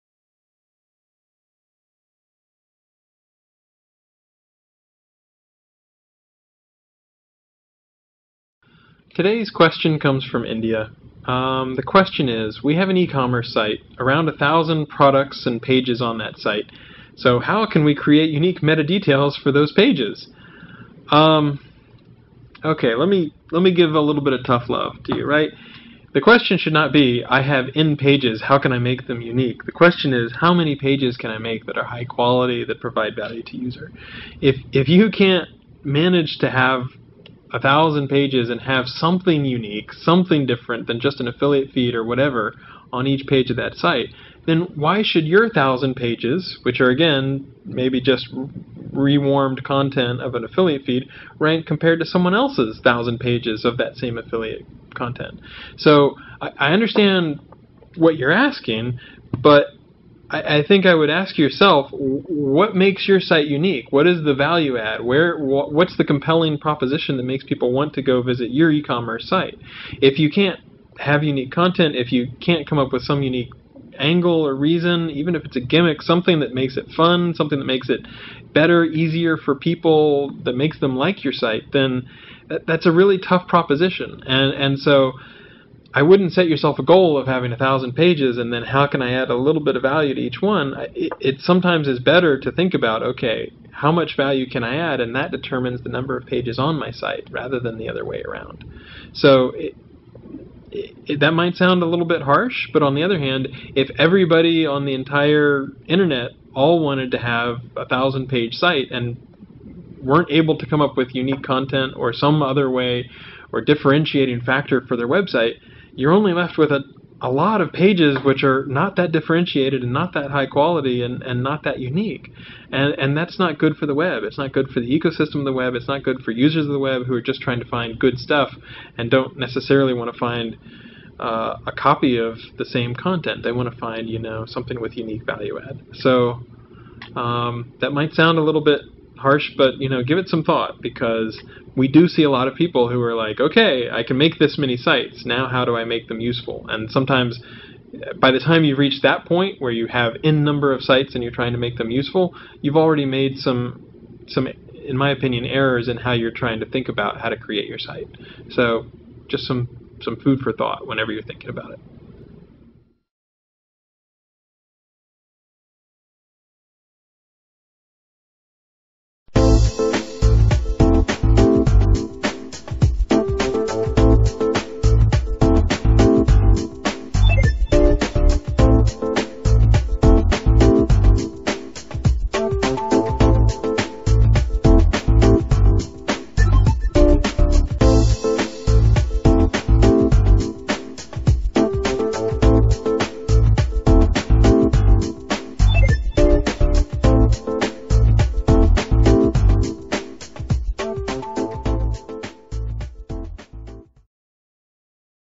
Páginas únicas | Posicionamiento Web
Seguro que vosotros también habéis visto webs enormes con muchas páginas y productos que proporcinan información obvia e incluso repetida. El objetivo de algunos webmasters es simplemente tener muchas páginas, pero sin embargo lo difícil es conseguir metadatos y páginas únicas que puedan diferenciarnos y mejorar nuestro posicionamiento web.

La respuesta de Matt Cutts al respecto nos ayuda a ver una vez más la posición de Google con respecto a la experiencia de búsqueda del usuario y a lo que espera de los creadores de webs, y lo que por tanto tendrá en cuenta en su posicionamiento web más tarde.

Para tener páginas únicas y con valor tenemos que centrarnos en ciertos factores de diferenciación: ¿qué las hace únicas? ¿qué les pueden proporcionar a nuestro usuario?

Configurad vuestra web con respecto al contenido que creáis valioso, no os pongáis un límite de páginas a alcanzar, porque entonces estarán vacías de contenido y no beneficiará nada a vuestro posicionamiento en el ranking.

Si tenéis dudas relacionadas con la creación de páginas valiosas y únicas podéis poneros en contacto con nosotros a través de este vídeo. También encontraréis más información sobre posicionamiento web